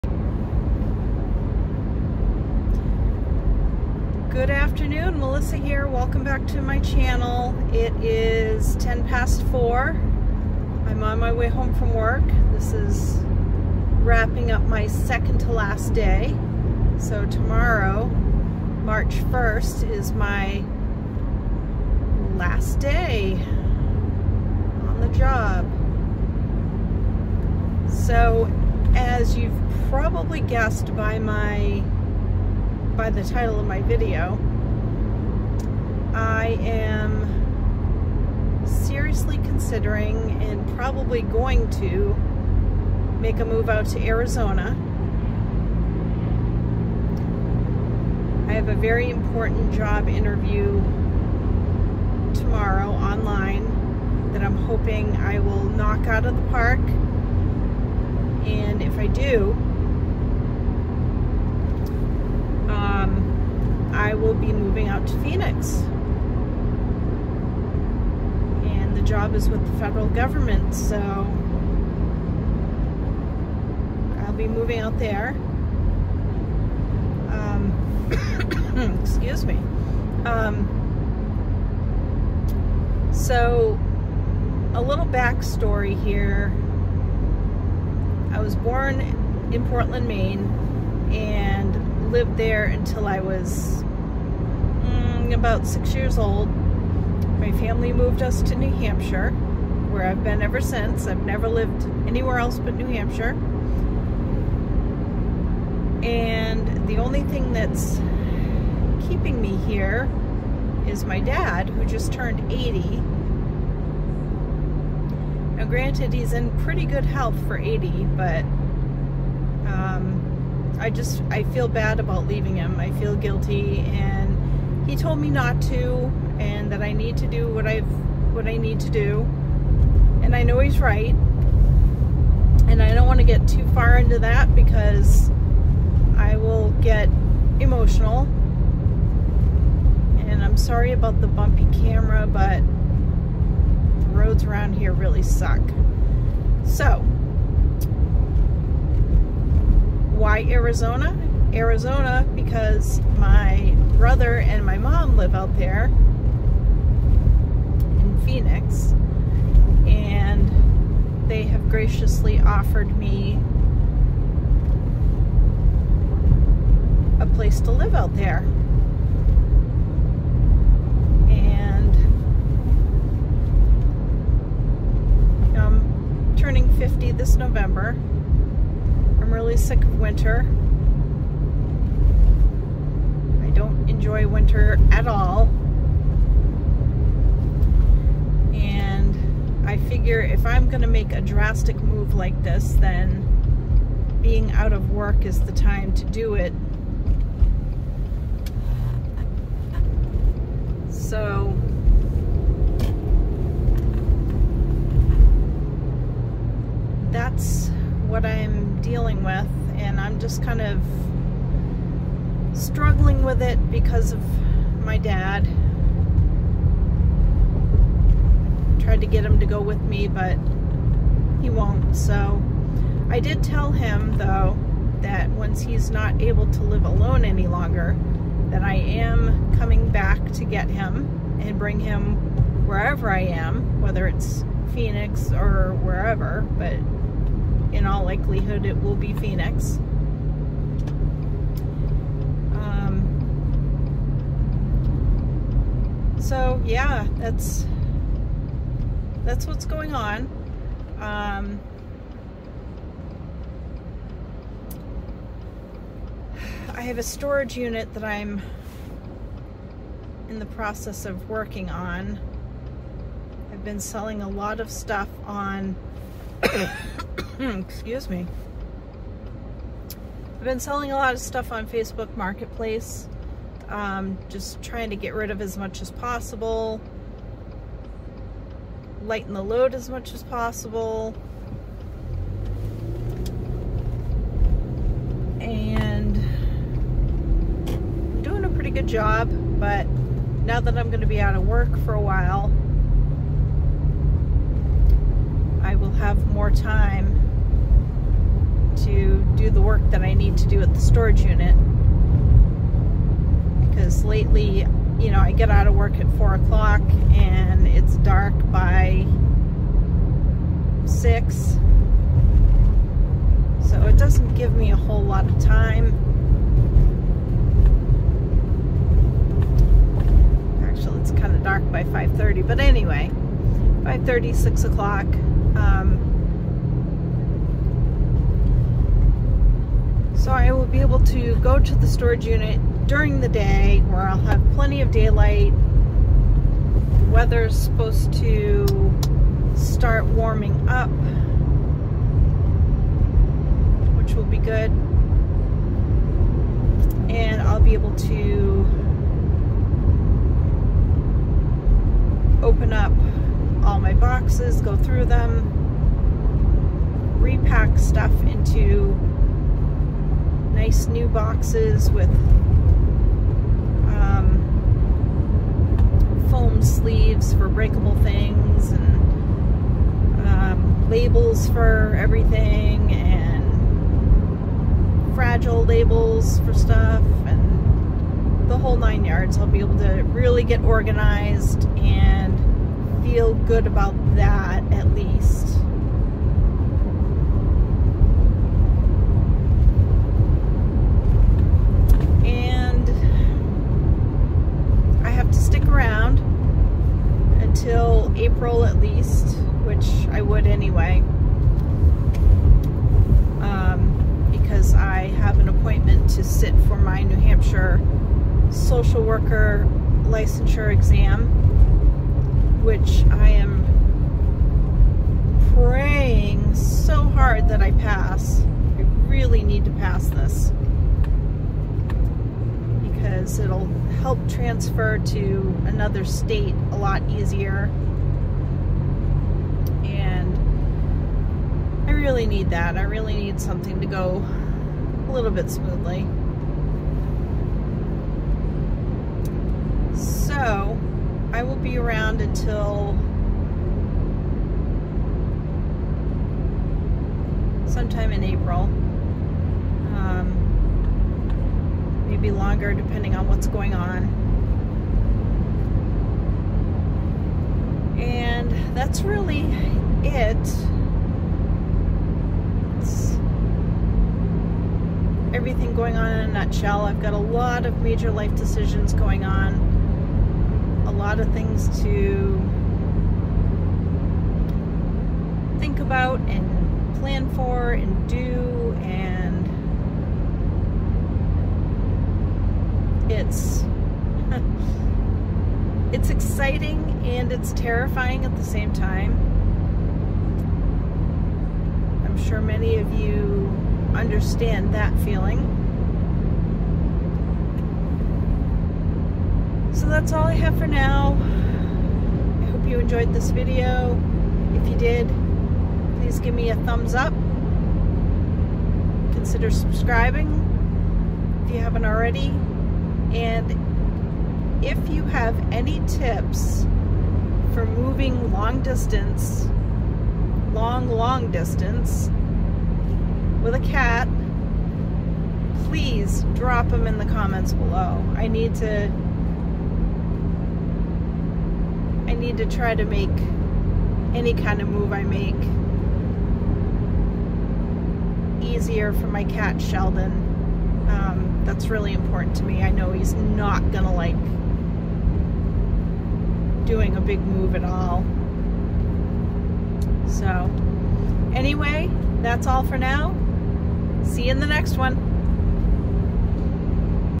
Moving to ARIZONA??
In this video, join me as I share my major life decision of potentially moving to Arizona. Will I make the move?